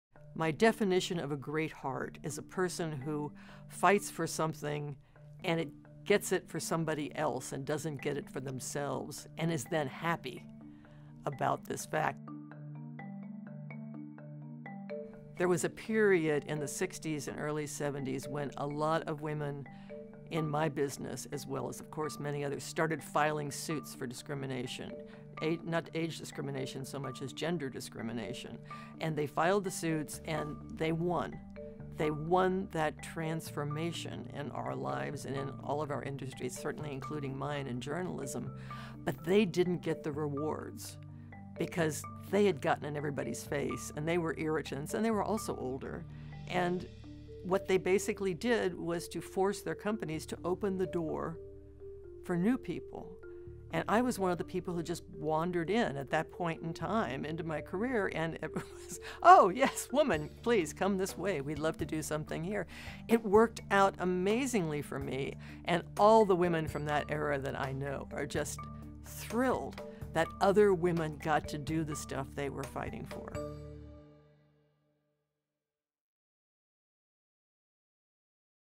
Weekend Essay | No Stopping Us Now: The Adventures of Older Women in American History
"My definition of a great heart is a person who fights for something, and gets it for somebody else, and doesn't get it for themselves, and is then happy about this fact," says New York Times columnist and author Gail Collins.